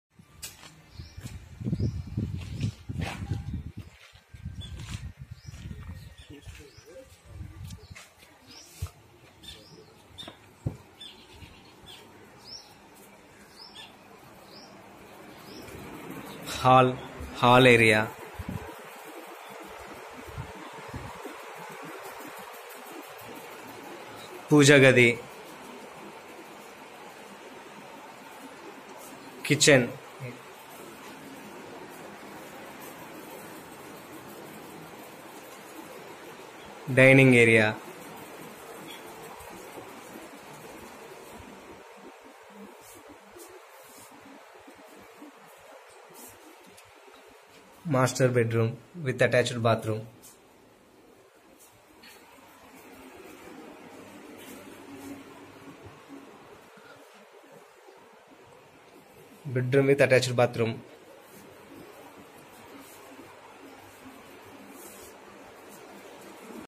68 lakhs house for sale@SSVSLR
*🤝68L Home for Spot Pay*

*👌Area Developments*

🚍100" బస్ రోడ్డు పక్కనే
🛣️ఔటర్ రింగ్ రోడ్  లోపల
🛣️సాగర్,విజయవాడ హైవే మధ్య..
🚇మెట్రో& ఆదిభట్ల (15min)
✈️ఎయిర్పోర్ట్ (30min)..

*మీ పెట్టుబడి & సిటీ అభివృద్ధి వేగంగా జరుగుతాయి👍*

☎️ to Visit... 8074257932